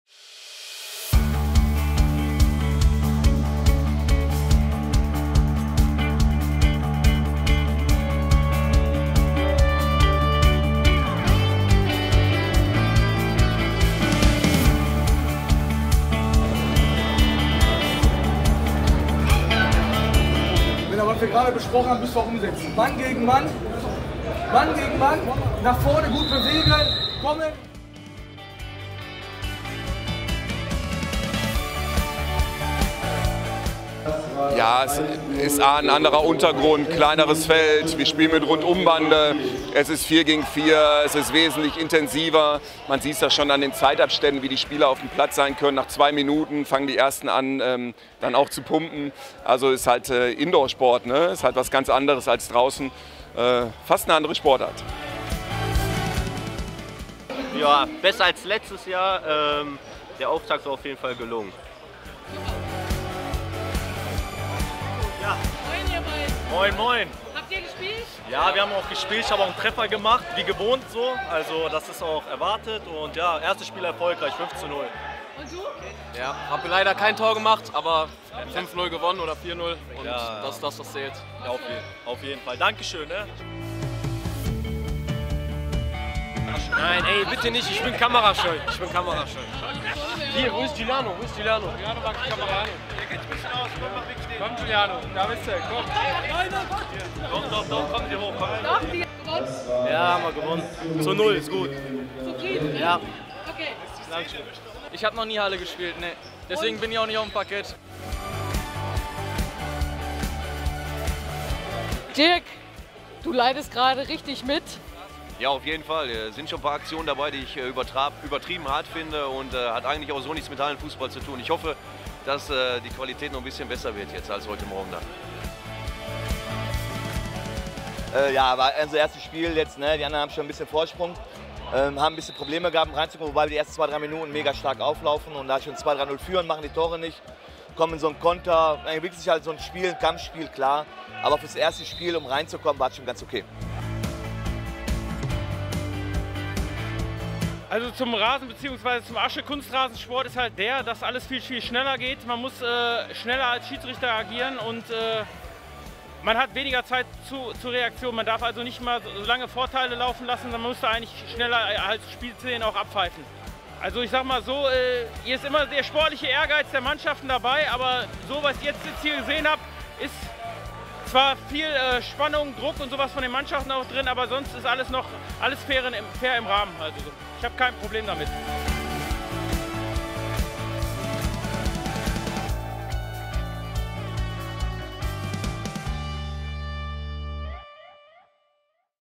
Zwischenrunde der Hallenstadtmeisterschaft 2024
Das war die Zwischenrunde der Stadtmeisterschaft im Hallenfußball in Essen 2024.